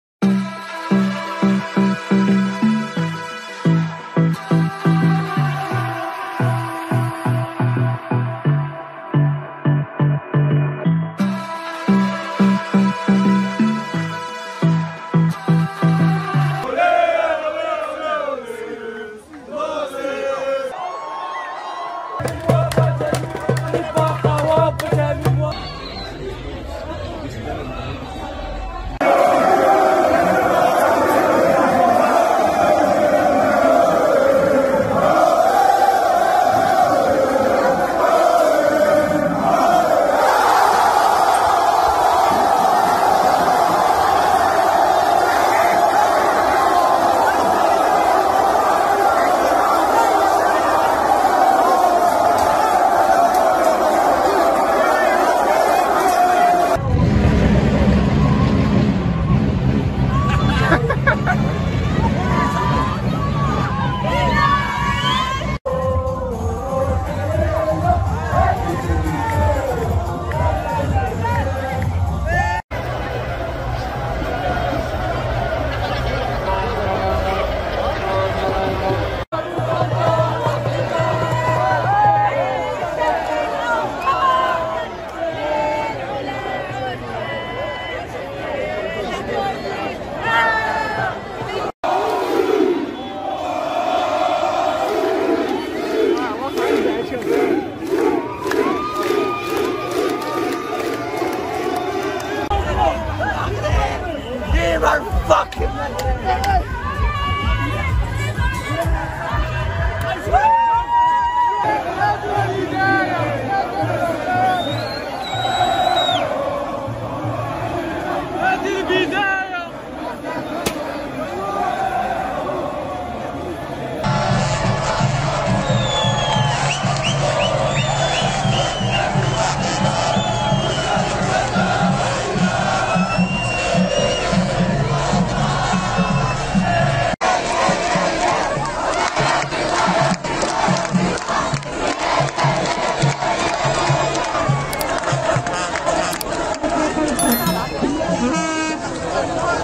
MOROCCO Vs Portugal | Crazy Morocco Fan Reactions To Win Against Portugal | Good BYE RONALDO!
Casablanca
Fans in Casablanca celebrate like CRAZY as Morocco beat Portugal
 first Arab and African country to ever reach the tournament’s semi-finals stage.
Completely Crazy Morocco Fan Reactions To Win Against Portugal In The World Cup

من قطر 🇶🇦 سجدة أبطال المغرب شكرا بعد الانتصار التاريخي على البرتغال 🇲🇦 تاريخ يكتب هنا